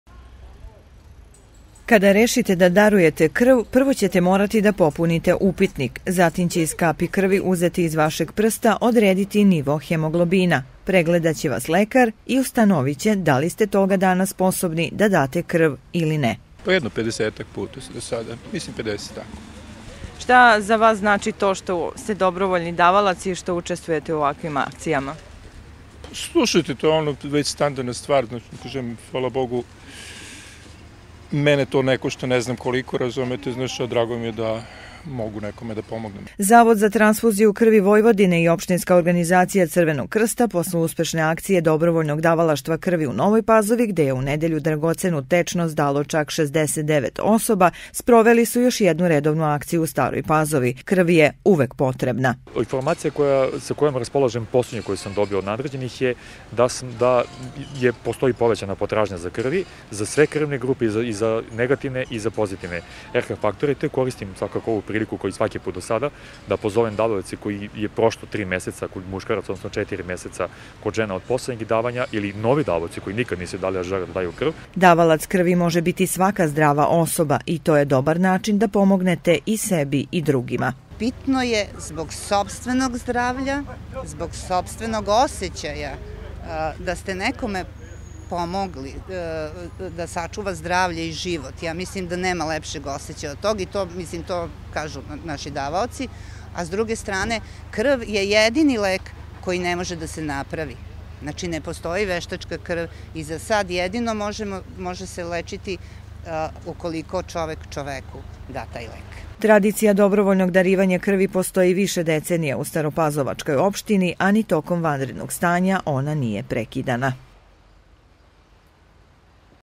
Zašto treba da date krv
Dobrovoljno davanje krvi nije samo humani gest kojim pomažete drugima. To je dobar način da pomognete i sebi.Još jedna akcija uspešno je održana u Staroj Pazovi .